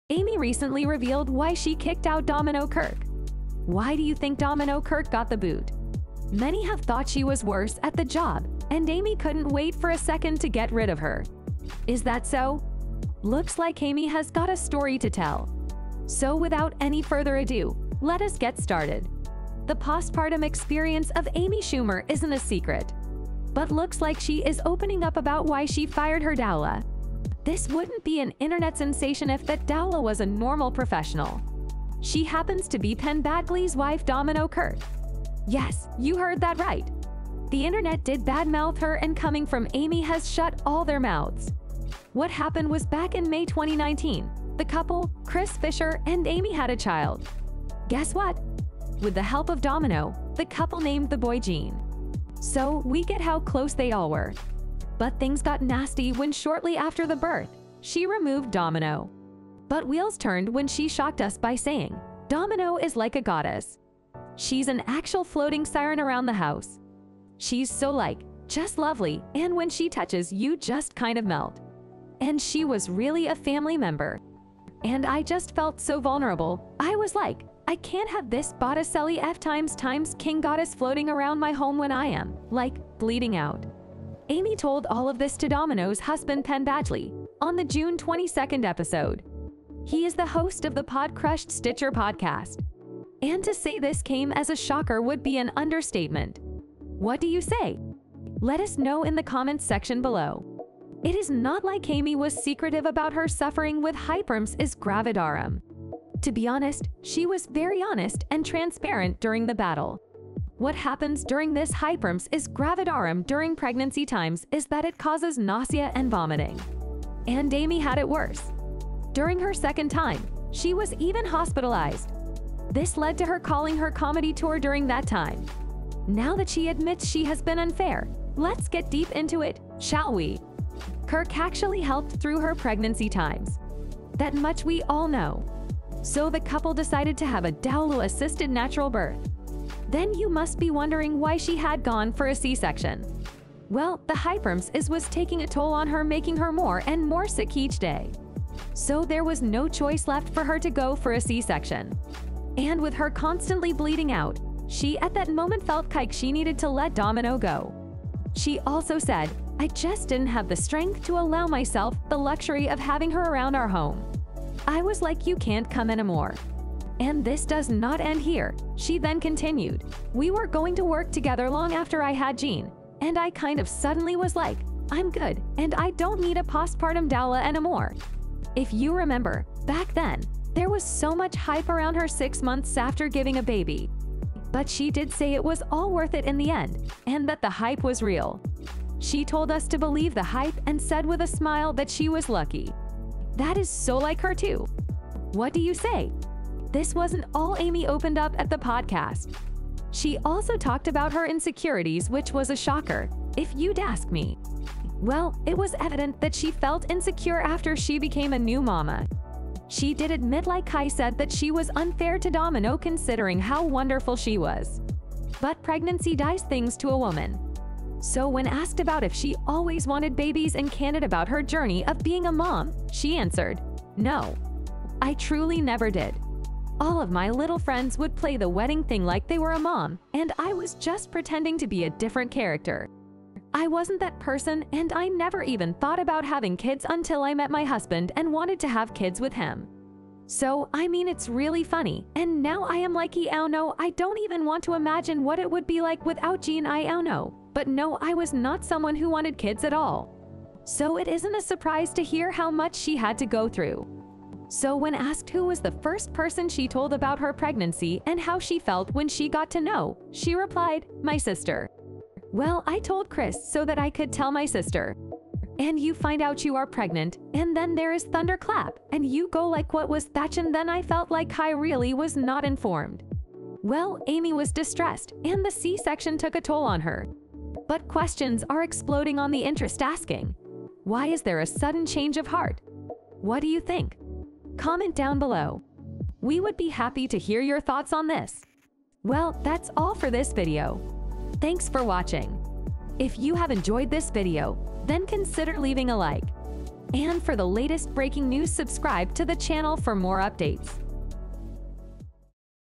Amy Schumer Shares Why Penn Badgley's Wife Domino Kirke was fired as her Doula.
Amy recently revealed why she kicked out Domino Kirke? Why do you think Domino Kirke got the boot? Many have thought she was worse at the job and Amy couldn't wait for a second to get rid of her. Is that so? Looks like Amy has got a story to tell.
The postpartum experience of Amy Schumer isn't a secret. But looks like she is opening up about why she fired her Doula. This wouldn't be an internet sensation if that Doula was a normal professional. She happens to be Penn Badgley's wife Domino Kirke. Yes, you heard that right. The internet did bad mouth her and coming from Amy has shut all their mouths.
So without any further ado, let us get started.